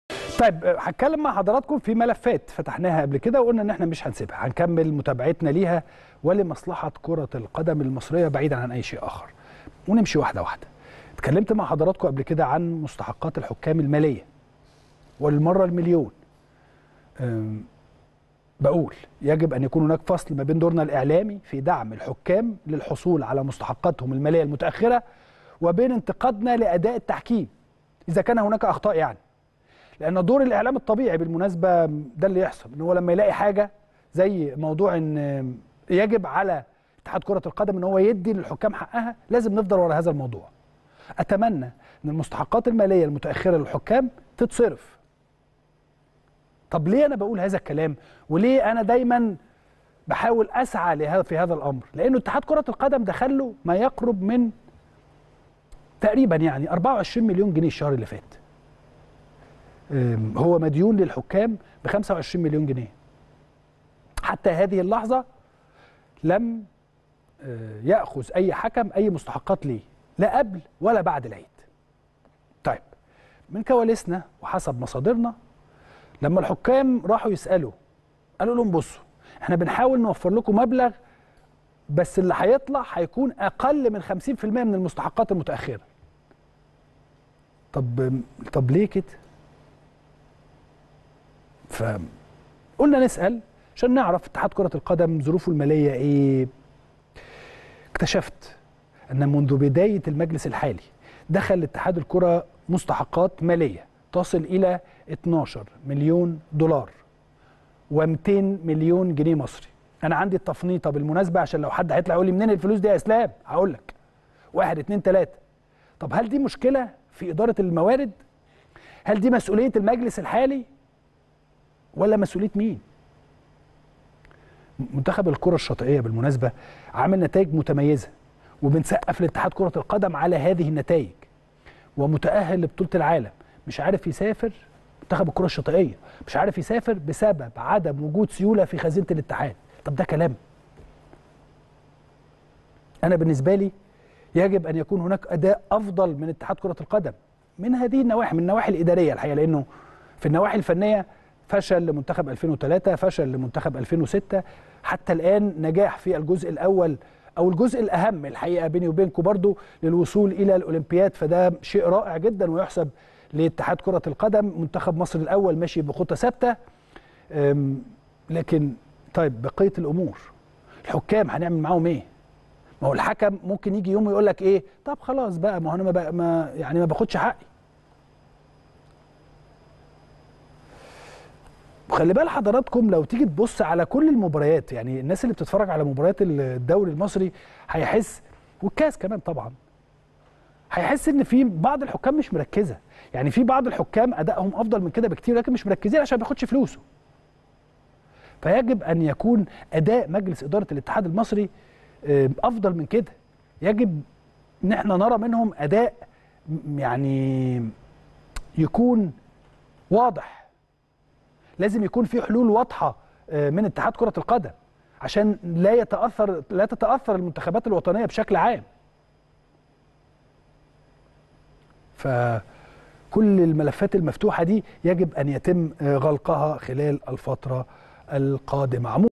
الشاطر: يجب أن نفصل ما بين دورنا الإعلامي لدعم الحكام للحصول على مستحقاتهم المالية وبين انتقادنا لهم
" من أجل مصلحة الكرة المصرية "
الشاطر: يجب أن يكون هناك فصل ما بين دورنا الإعلامي لدعم الحكام للحصول على مستحقاتهم المالية السابقة وبين انتقادنا لأخطائهم التحكيمية